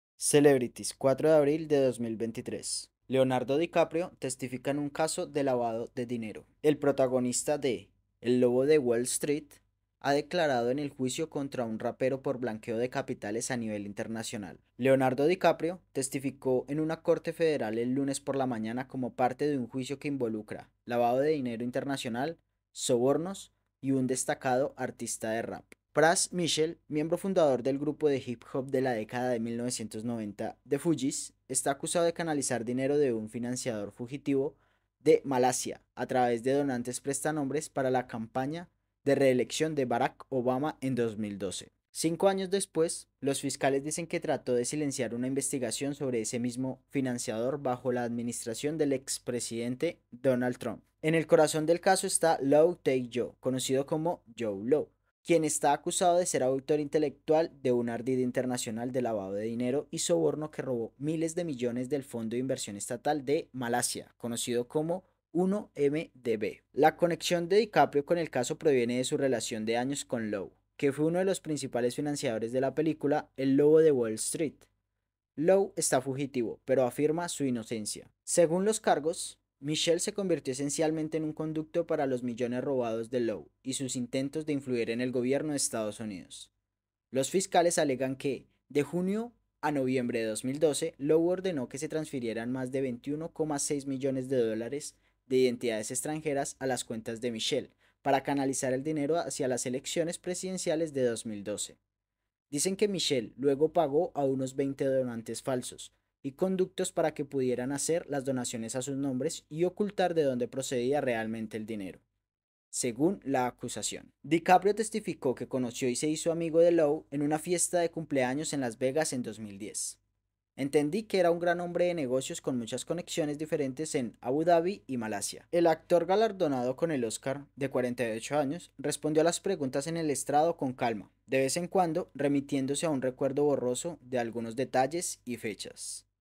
Leonardo Dicaprio testifica en un caso de lavado de dinero /Global.
Después de Dicaprio, varios testigos declararon que Michel se había acercado a ellos para hacer contribuciones ocultas a la campaña de Obama.
Richard Kromica, un banquero de inversiones, dijo que Michel le dijo que había llegado a su limité legal de contribución y le pidió a Kromica y a su esposo Joseph que hicieran una donación en su nombre.
Kromica dijo que Michel envio a la pareja $80.000 dólares para donar.